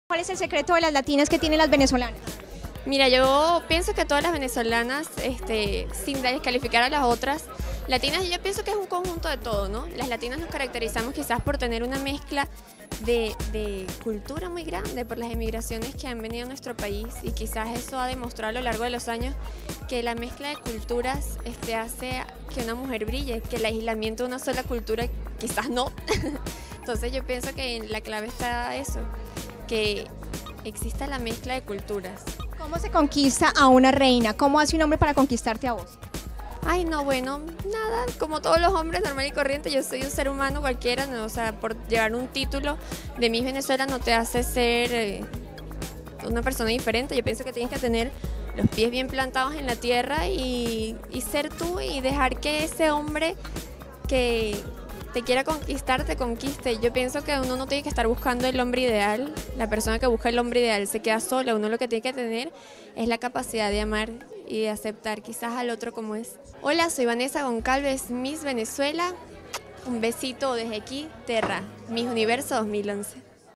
Miss Universo 2011 Miss Venezuela dice 'Tienes que mantener los pies sobre la tierra'
Vanessa Goncalvez, una de las candidatas más fuertes para ganar el Miss Universo dice sentirse como cualquier latina y mujer en general.